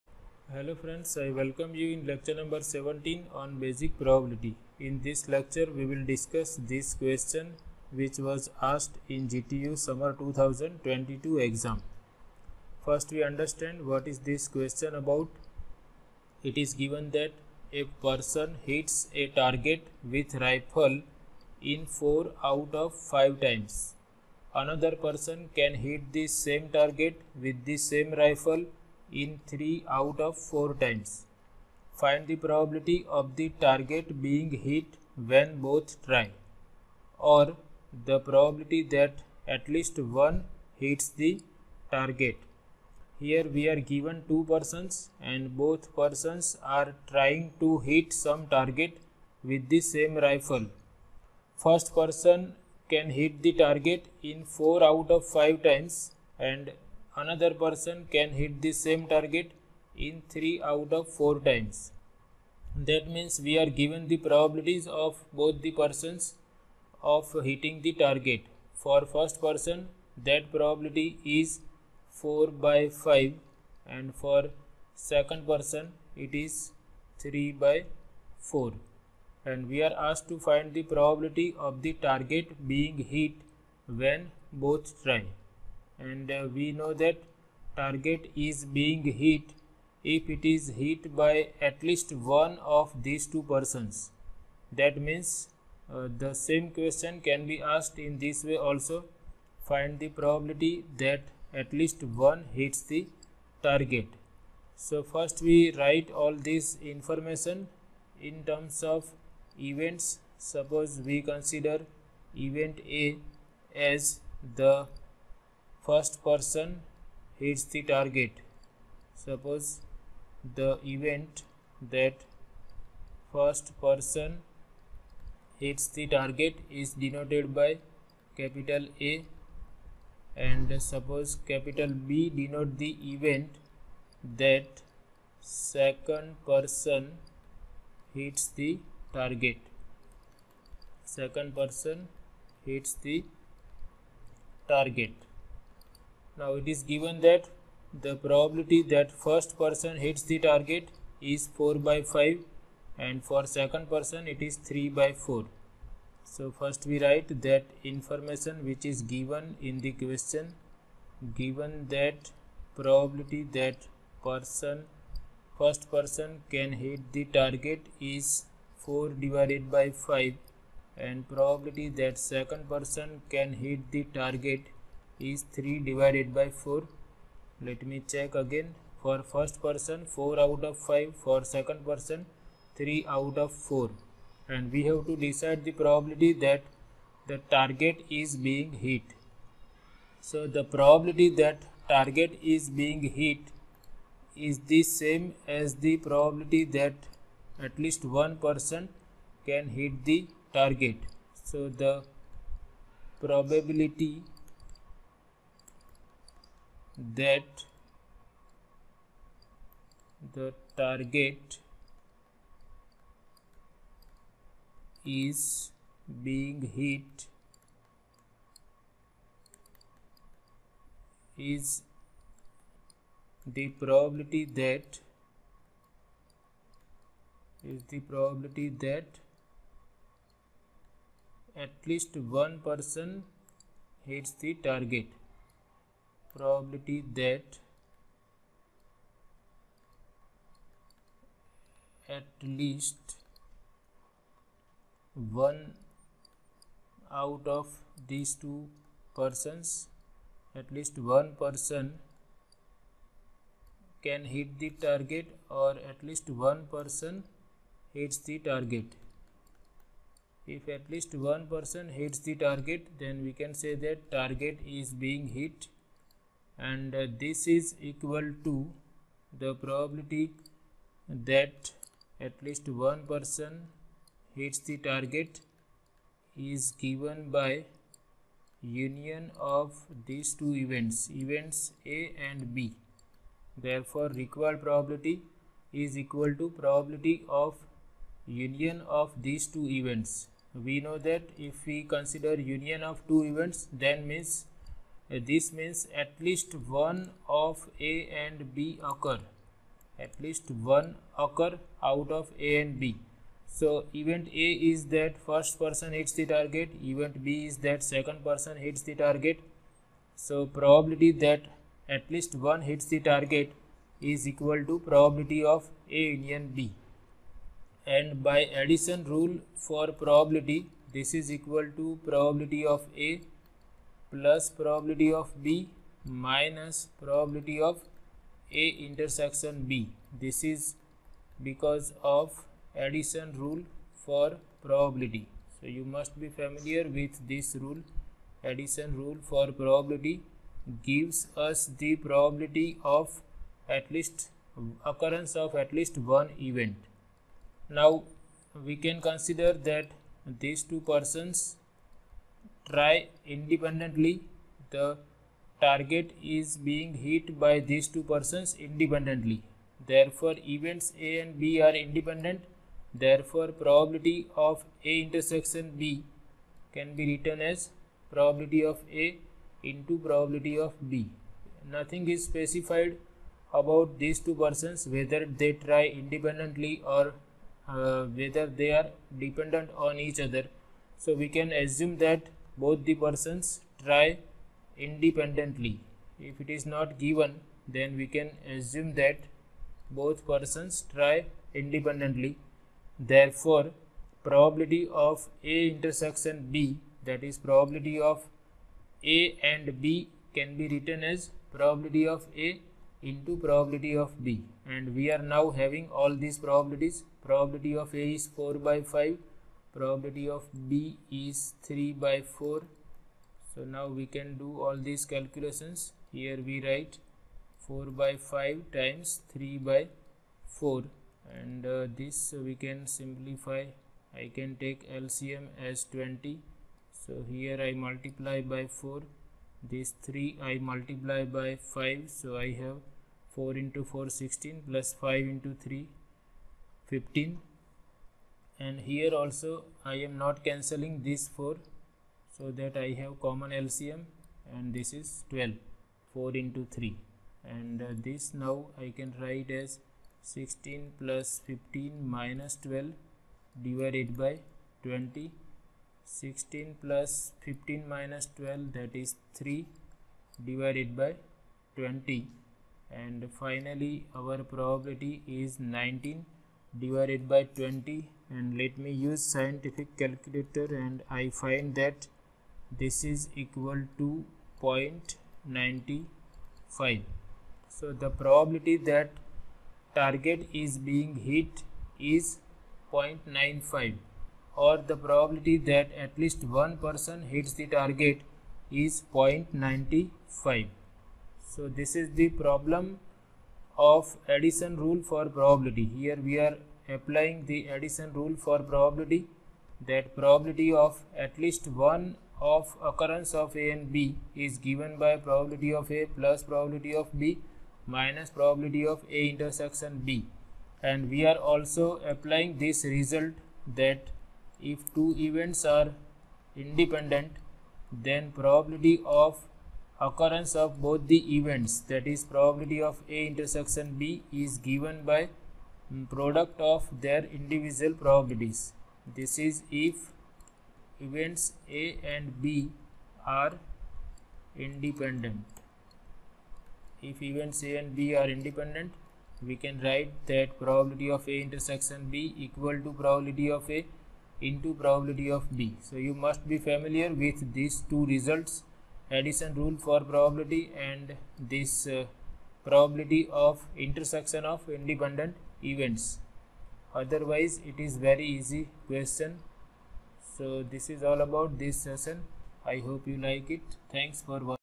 Lec17/Basic Probability/GTU Exam Questions
One more problem on basic probability is being discussed in this lecture.  The problem was asked in GTU Summer 2022 Exam.